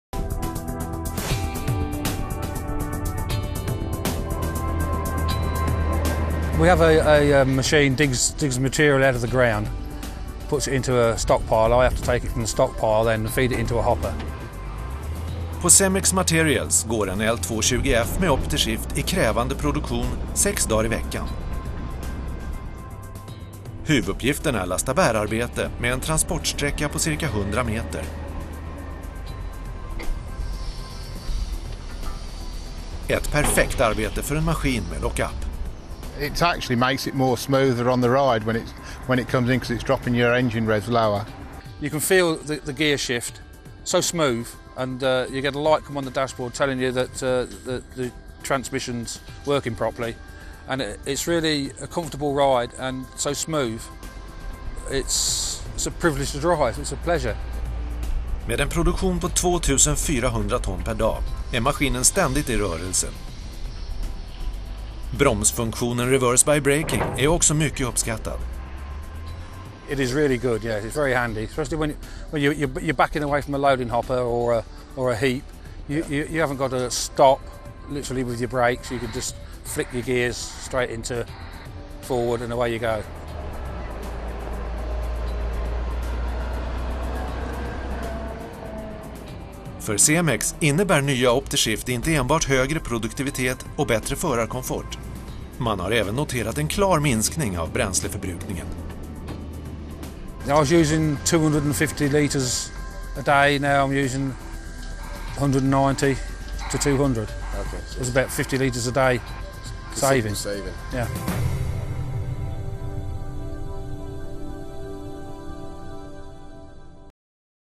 Volvo OptiShift - 20% lägre förbrukning hos Cemex Materials Ltd., England
Cemex rapporterar en sänkning från 250 liter/dag till 190-200 liter. Det är en sänkning med 20% med en Volvo L220F med OptiShift jämfört med en konventionell.  Hos Cemex går OptiShift lastmaskinen i lasta/bär ca 100 m transport. Perfekt för en maskin med lock up. Cemex lastar ca 2400 ton/dag. Med OptiShift får de dramatiskt lägre bränsleförbrukning, högre produktivitet och förarkomfort.